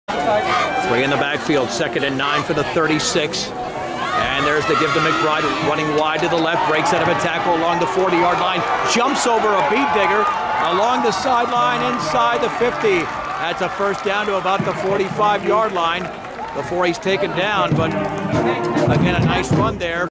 9-4-15 - 19 more yards for Toby McBride
Brush at Fort Morgan football on September 4, 2015. Fort Morgan defeated Brush, 47-7 (John Beltran on 1010 KSIR in Fort Morgan, Colorado